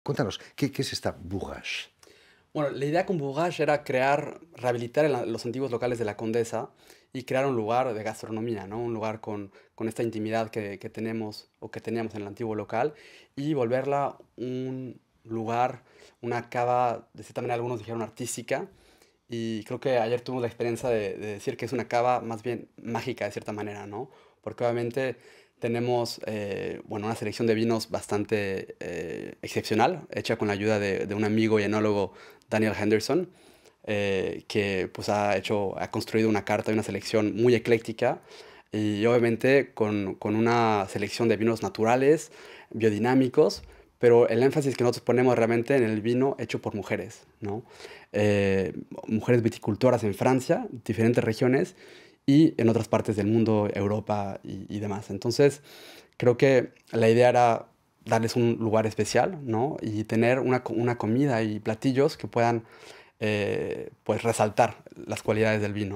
Un chef mexicano abre una vinoteca en Paris con vino hecho por mujeres
El chef mexicano Indra Carrillo (una estrella Michelin por su restaurante parisino ‘La Condesa’) acaba de abrir en París una vinoteca que pone el acento en el vino producido por mujeres.